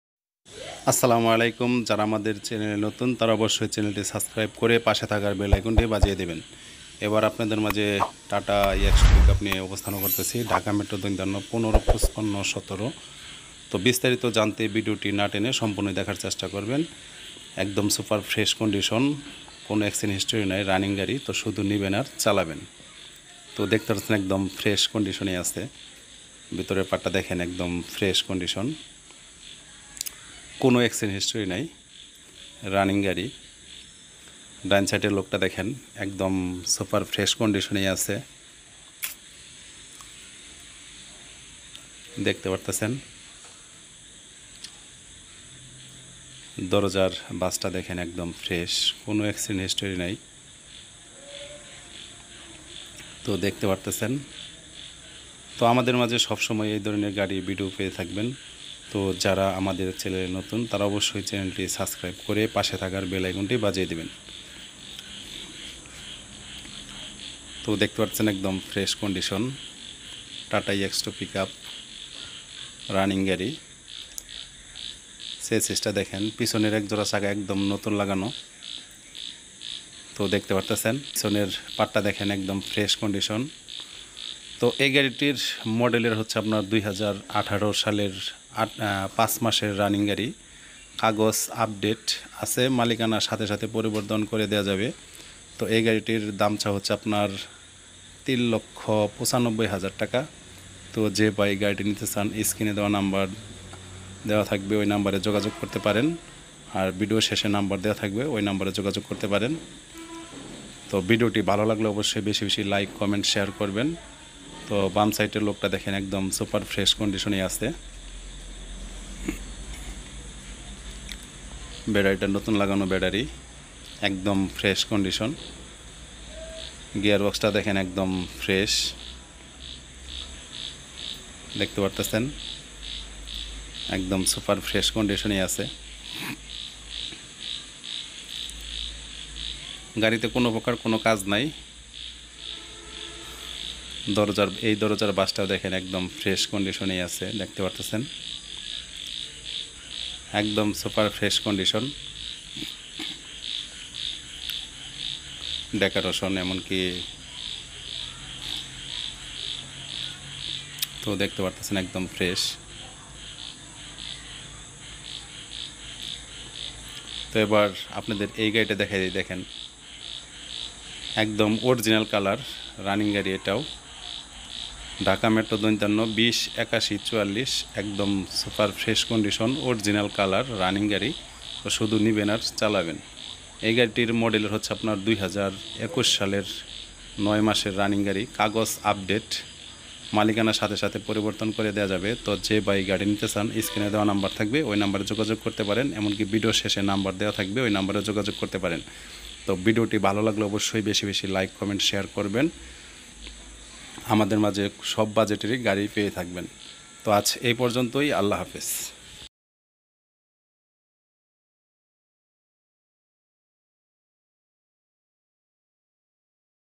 ফ্রেশ কন্ডিশন Tata Ex2 Pickup গাড়ীটি বিক্রি করা হবে.Model-2018-05 মাসের রানিং গাড়ি.কাগজ আপডেট আছে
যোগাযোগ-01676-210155